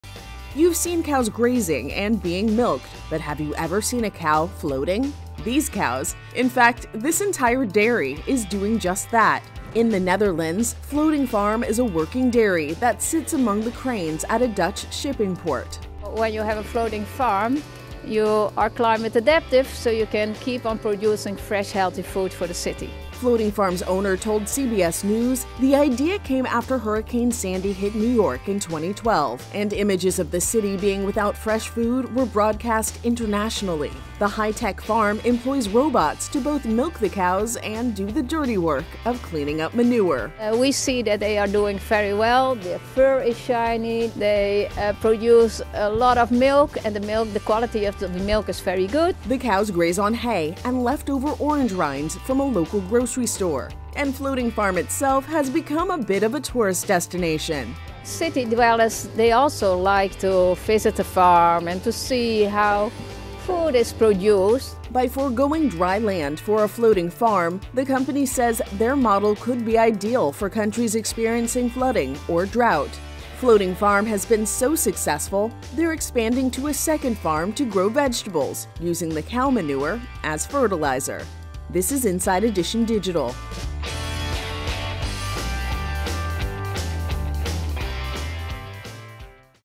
Cows Float on This Innovative Dairy Farm
In the Netherlands, Floating Farm is a working dairy that sits among the cranes at a Dutch shipping port. The high-tech farm employs robots to both milk the cows and to do the dirty work of cleaning up manure. By forgoing dry land for a floating farm, the company says their model could be ideal for countries experiencing flooding or drought. Floating Farm has been so successful, they’re expanding to a second farm to grow vegetables, using the cow manure as fertilizer.